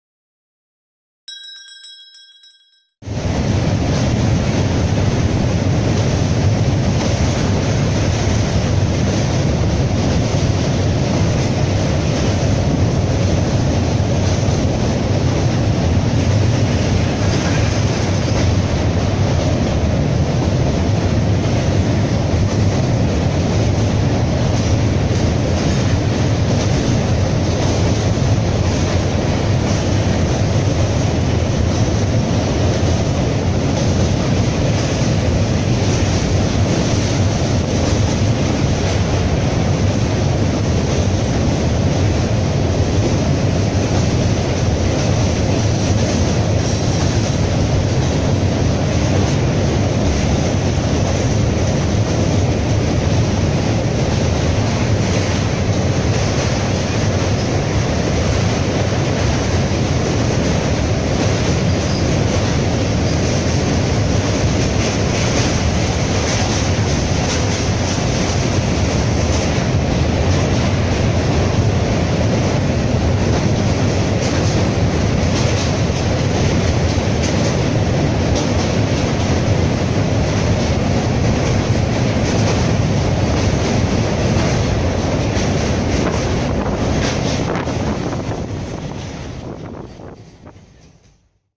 The Barhamputra River Bridge In Assam.
A full view of the Barhamputra River Bridge In Assam.A view from. Train